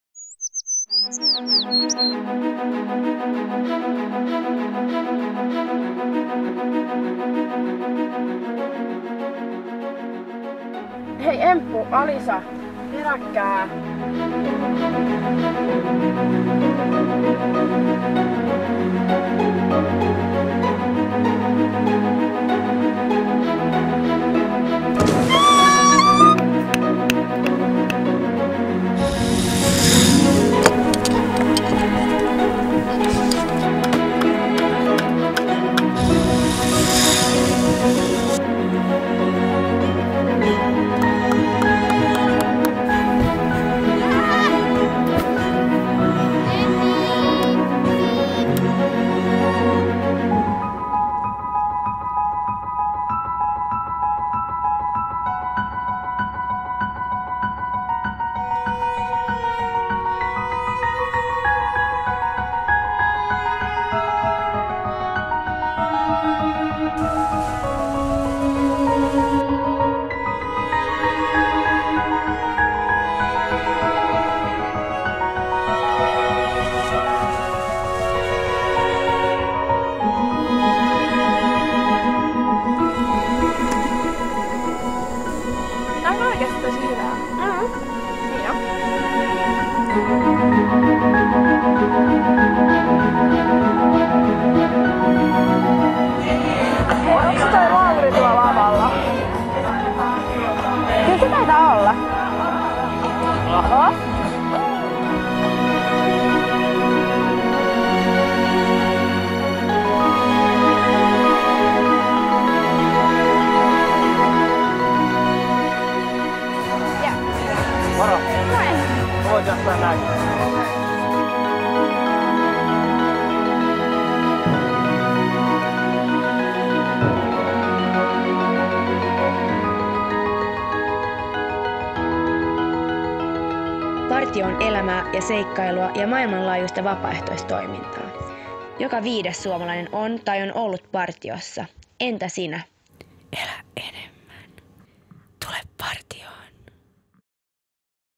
Lev mer - Bli scout
Scouting är liv och äventyr och ett världsomspännande frivilligarbete. Det är en rolig, mångsidig och internationell hobby fylld av vänskap, skratt och oförglömliga upplevelser.

Scouterna är öppna för alla och du kan komma med i vilken ålder som helst, också som vuxen! Läs mer på

Produktion: Suomen Partiolaiset - Finlands Scouter ry
Regi, bild, klipp: Aapo Rainio
AD: Jussi Peso
Tack till: Petra, Aino, Lauri, Antti, Ilves 2013 - distriktsläger och Satahanka sjöscoutlägret